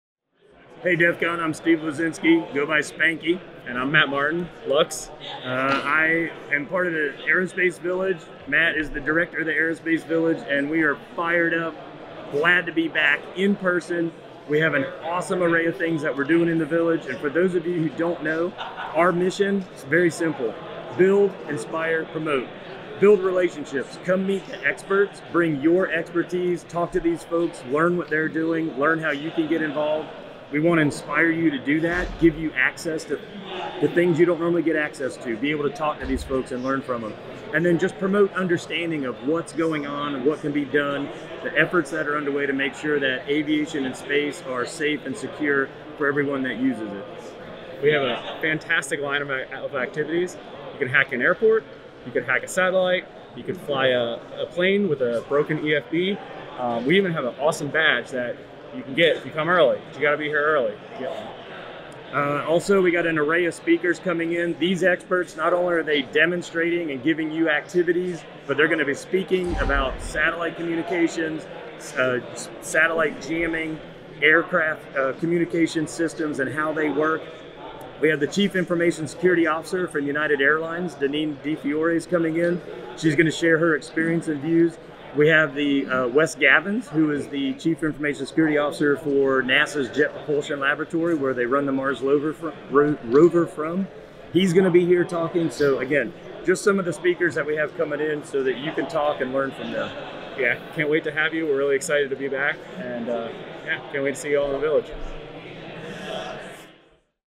DEF CON 30 - Aerospace Village Interview
Interviewer Ry Boken gets the scoop on what Aerospace Village has in store for DEF CON 30.